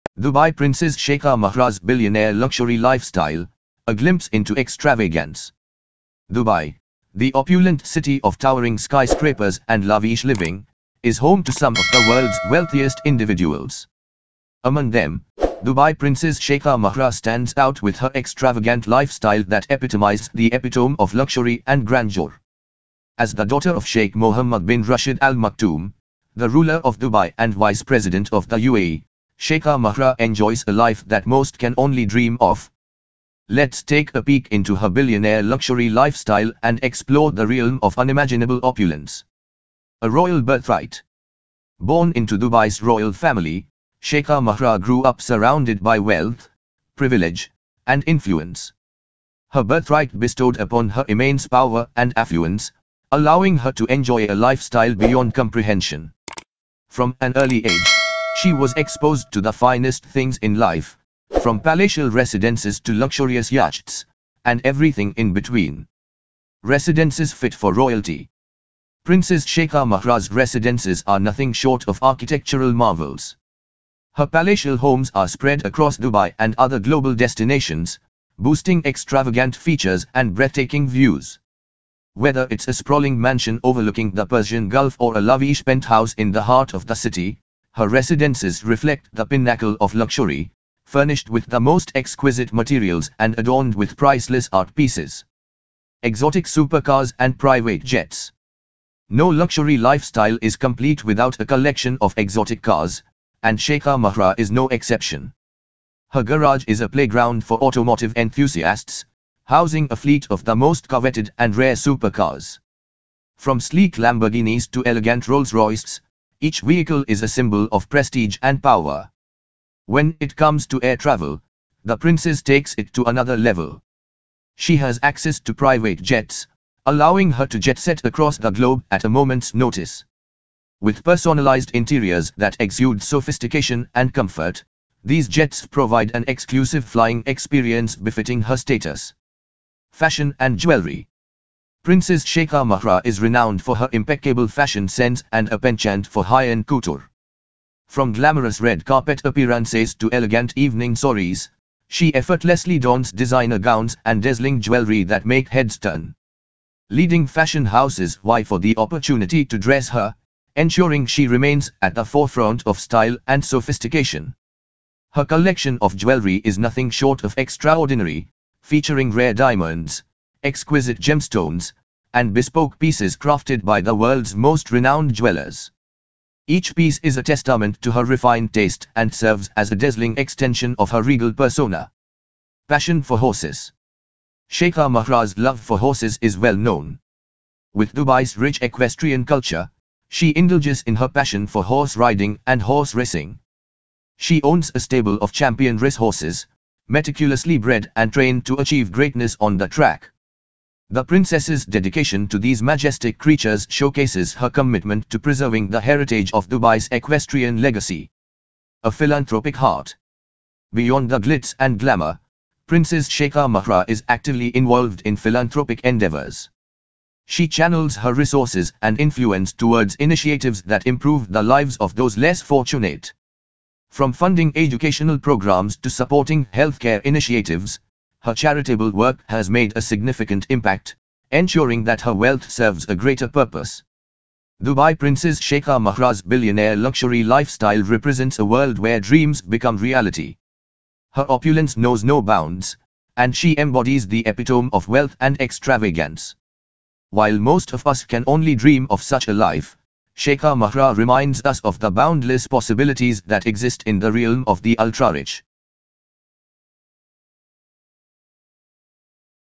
Dubai Princess Sheikha Mahra's Billionaire Luxury Lifestyle
Dubai Princess Sheikha Mahra's Billionaire Luxury Lifestyle: A Glimpse into Extravagance

Dubai, the opulent city of towering skyscrapers and lavish living, is home to some of the world's wealthiest individuals. Among them, Dubai Princess Sheikha Mahra stands out with her extravagant lifestyle that epitomizes the epitome of luxury and grandeur. As the daughter of Sheikh Mohammed bin Rashid Al Maktoum, the ruler of Dubai and Vice President of the UAE, Sheikha Mahra enjoys a life that most can only dream of. Let's take a peek into her billionaire luxury lifestyle and explore the realm of unimaginable opulence.